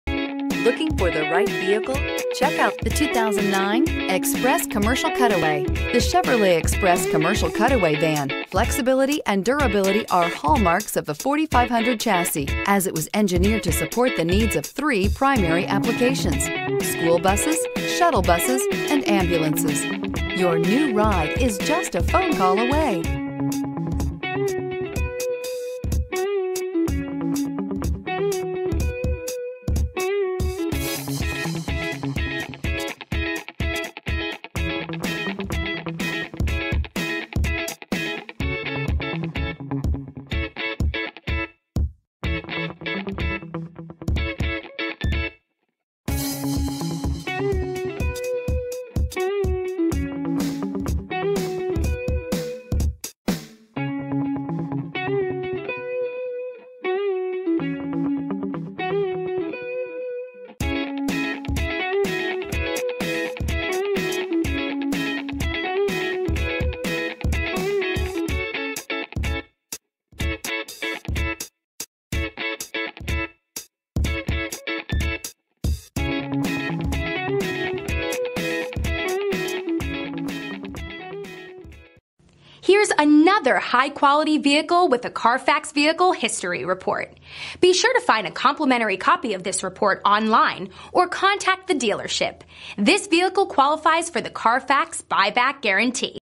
2009 Chevrolet Express Cutaway T0081
2009 Chevrolet Express Cutaway 16' Box Truck

2 owner, overall clean and well maintained 16' box truck ready to work!